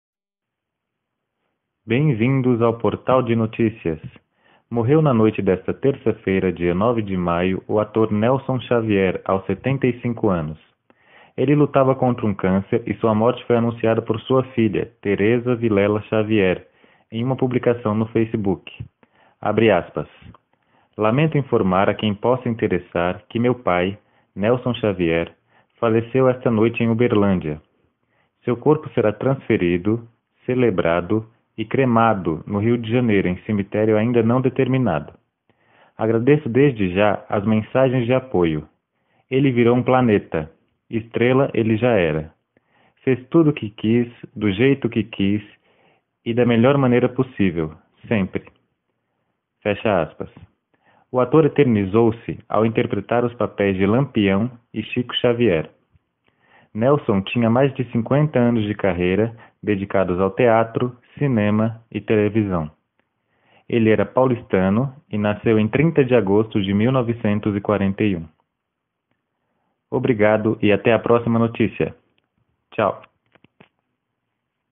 Morre ator Nelson Xavier
Morreu na noite desta terça-feira dia 09 de maio o ator Nelson Xavier aos 75 anos. Ele lutava contra um câncer e sua morte foi anunciada por sua filha, Tereza Villela Xavier, em uma publicação no Facebook: ''Lamento informar a quem possa interessar que meu pai, Nelson Xavier, faleceu esta noite em Uberlândia. Seu corpo será transferido, celebrado e cremado no Rio de Janeiro em cemitério ainda não determinado. Agradeço desde já as mensagens de apoio. Ele virou um planeta! Estrela ela já era. Fez tudo o que quis, do jeito que quis e da sua melhor maneira possível, sempre''.
O ator eternizou-se ao interpretar os papéis de Lampião e Chico Xavier. Nelson tinha mais de 50 anos de carreira dedicados ao teatro, cinema e televisão.
O ator era paulistano e nasceu em 30 de agosto de 1941.